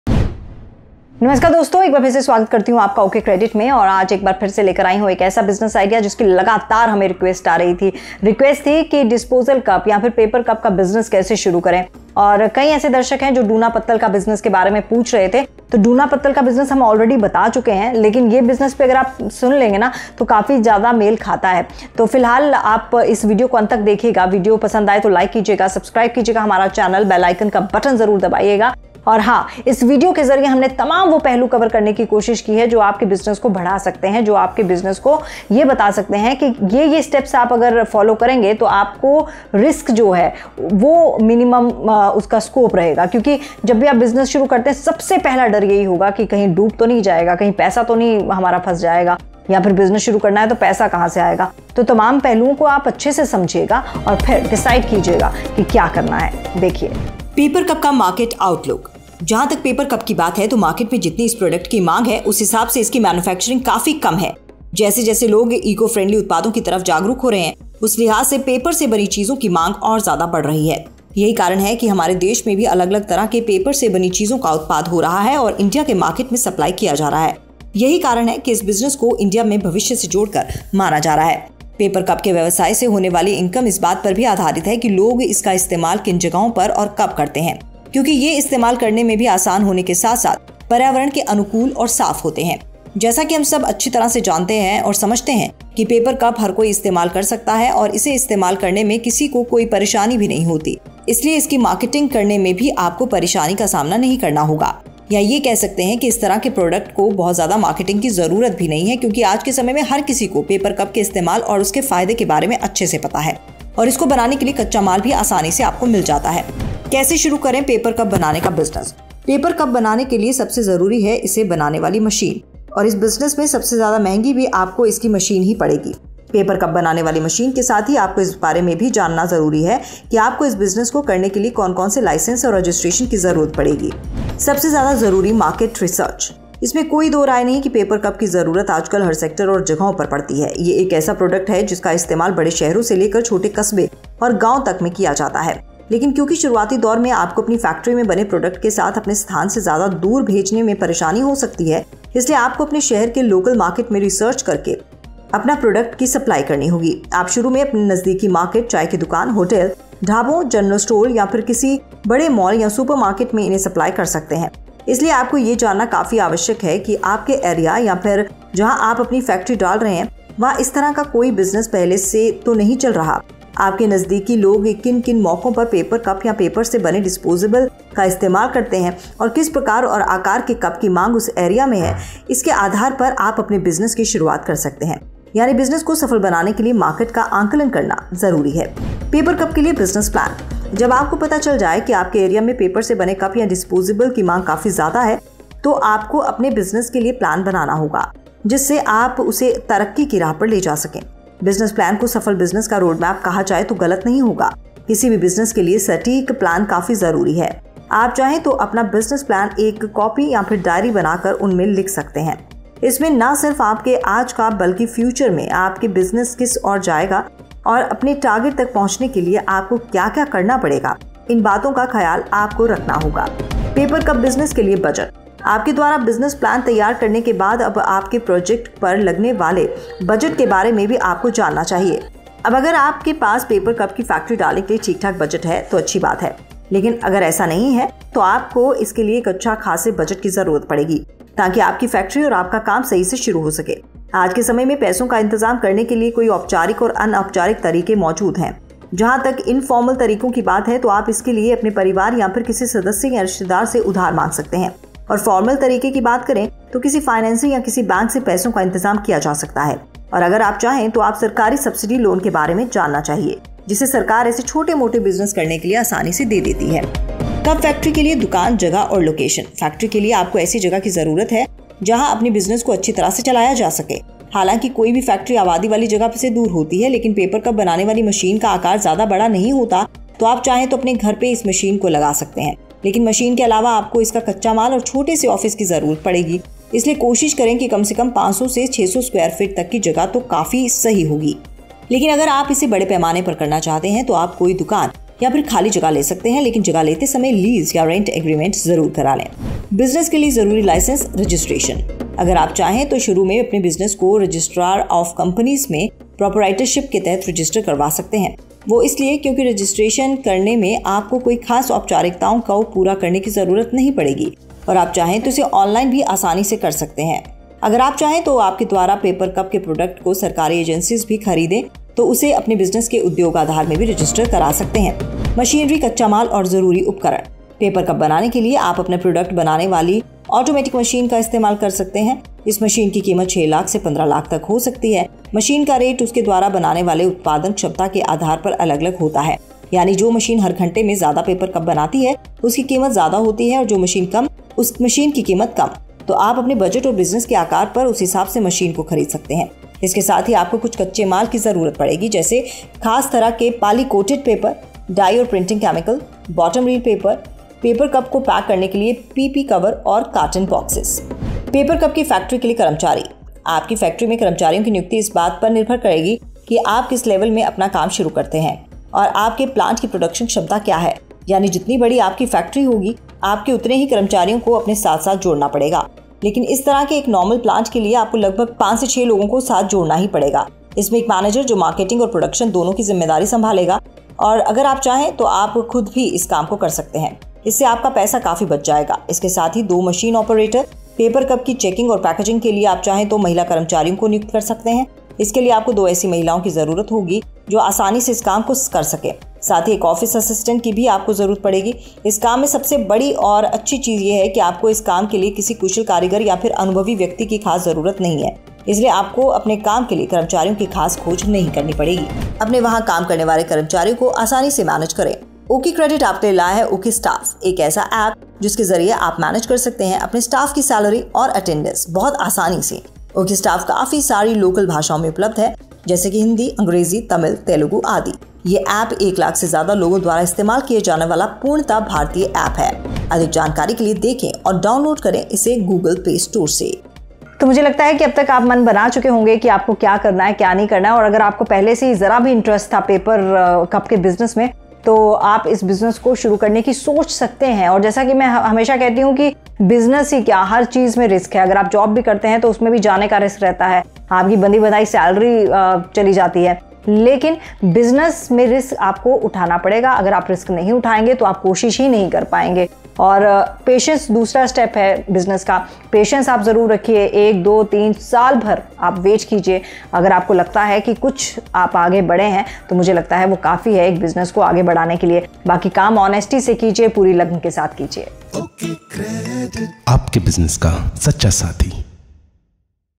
How to Start a Paper Cup Business? | पेपर कप बनाने का बिजनेस कैसे शुरू करें? | OkCredit
पेपर कप की बात करें तो ये खाने में इस्तेमाल की जाने वाले डिस्पोजेबल आइटम में एक काफी बड़े पैमाने पर इस्तेमाल की जाने वाली आइटम है. और डिस्पोजेबल उत्पादों का उपयोग में लाने का सबसे बड़ा कारण किसी पार्टी, शादी, समारोह में इस्तेमाल करना या फिर सार्वजनिक स्वास्थ्य को बढ़ावा देना ही था.
पेपर से बने डिस्पोजेबल के साथ सबसे अच्छी बात ये है की ये मिट्टी में आसानी से नष्ट हो जाने हैं जिससे ये हमारे वातावरण को गंदा भी नहीं करते. पिछले कुछ सालों में हमारे वातावरण को होने वाले खतरे को देखते हुए सभी देशों ने पृथ्वी के संरक्षण के लिए मजबूत कोशिश की है. और उन्ही कोशिशों की बदौलत पेपर कप के उत्पाद की उत्पति की गई.
पेपर से बने कप या बाकि डिस्पोजेबल की दूसरी सामग्री से बने डिस्पोजेबल की तुलना में कई फायदे हैं. क्योंकि इन्हें देखने में स्टाइलिश, सुंदर और जल्दी नष्ट हो जाने के लिए बनाया है. इसलिए दुनियाभर में इसका काफी इस्तेमाल किया जाता है.

पेपर कप को बड़ी ही आसान प्रक्रिया से बनाया जाता है. और इस पेपर कप या पेपर से बने बाकि डिस्पोजेबल को बनाने में ज्यादा कचरा भी नहीं पैदा होता. और इन्हें दूबारा रीसायकल करना भी काफी आसान काम है.

 पेपर कप की सबसे बड़ी खासियत ये है की इसमें अपने हिसाब से कस्टम डिजाइन बड़ी ही आसानी से की जा सकती है. इसलिए कंपियां अपने प्रोडक्ट की मार्केटिंग के लिए भी इन पर कंपनी का लोगो और जरूरी चीज डिजाईन कराना पसंद करती है.
पेपर कप या पेपर से बने डिस्पोजेबल बाजार में कई डिजाईन, रंग, बनावट और आकार में बिकते हैं. इसलिए ये दिखने में स्टाइलिश, खूबसूरत और शानदार भी होते हैं. और अगर आप पेपर कप की मैनुफैक्चरिंग करना चाहते हैं तो आपको इसके बारे में सभी जरूरी चीजों को जानना काफी जरूरी है |

आप के द्वारा पेपर कप बिजनेस से जुड़े कुछ जरूरी सवाल :

सवाल- क्या घर में पेपर कप के बिजनेस को शुरु किया जा सकता है ?

जवाब- पेपर कप बनाने वाली मशीन का आकार ज्यादा बड़ा नहीं होता है और आप चाहें तो इसे आपके घर में या किसी छोटे कमरे में भी लगा सकते हैं. लेकिन मशीन के अलावा आपको इसका कच्चा माल और छोटे से ऑफिस की भी जरूरत पड़ेगी. इसलिए कोशिश करे की कम से कम 500 से 600 Sq.ft.  तक की जगह हो तो काफी सही रहेगा. लेकिन अगर आप इसे बड़े पैमाने में करना चाहते हैं तो आप कोई दुकान या फिर खाली जगह ले सकते हैं.

सवाल- क्या भारत में पेपर कप का बिजनेस करना सही रहेगा?

जवाब- मार्केट में जितनी इस प्रोडक्ट की मांग है उस हिसाब से इसकी मैन्युफैक्चरिंग काफी कम है जैसे जैसे लोग इको फ्रेंडली उत्पादों की तरफ जागरुक हो रहे हैं उस लिहाज से पेपर से बनी चीजों की मांग और ज्यादा बढ़ रही है. यही कारण है की हमारे देश में भी अलग अलग तरह के पेपर से बनी चीजों का उत्पाद हो रहा है और इंडिया के मार्केट में सप्लाई किया जा रहा है. यहीं कारण है की इस बिजनेस को इंडिया में भविष्य से जोड़ कर माना जा रहा है और आज के समय में इस तरह का व्यापार शुरु कर के आप भी इन प्रोडक्ट का इस्तेमाल करने वालों की जरूरतों को पूरा कर सकते हैं.

सवाल- क्या पेपर कप का बिजनेस शुरु करने के लिए सरकार सब्सिडी लोन देती है?

जवाब- आज के समय में पैसों का इंतजाम करने के कई फॉर्मल और इनफॉर्मल दोनों तरीके मौजूद है. जहां तक इनफॉर्मल तरीकों की बात है तो आप इसके लिए अपने परिवार या फिर किसी सदस्य या रिश्तेदार से उधार मांग सकते हैं. और फॉर्मल तरीकों की बात करें तो आप किसी फाइनेंसर या किसी बैंक से पैसों का इंतजाम कर सकते हैं. और अगर आप चाहें तो आप सरकारी की तरफ से आने वाली तरह तरह के सब्सिडी लोन ले सकते हैं. जिसे सरकार ऐसे छोटे मोटे बिजनेस करने के लिए आसानी से दे देती है. केंद्र सरकारों के साथ ही कई राज्य की सरकारें भी अपने राज्य के लोगों के रोजगर या बिजनेस करने के लिए स्कीम निकालती है. आप इनका फायदा उठा कर अपना बिजनेस कर सकते हैं.

सवाल- पेपर कप बनाने वाली मशीन कहां से मिलेगी और इसकी कीमत कितनी है?

जवाब- आपके राज्य के किसी भी बड़े शहर से ये मशीन मिल जाएगी. लेकिन अगर आपको अपने एरिया में ये नहीं मिल रही है तो आप दिल्ली मुंबई या फिर चंडीगढ़ से इसे खरीद सकते हैं. मशीन की कीमत 6 लाख से 15 लाख तक हो सकती है मशीन का रेट उसके द्वारा बनने वाले उत्पादन क्षमता के आधार पर अलग अलग होती है. यानी जो मशीन हर घंटे में ज्यादा पेपर कप बनाती है उसकी कीमत ज्यादा होती है और जो मशीन कम पेपर कप तैयार करती है उसकी कीमत कम. तो आप अपने बजट और बिजनेस के आकार के हिसाब से इन मशीन को खरीद सकते हैं |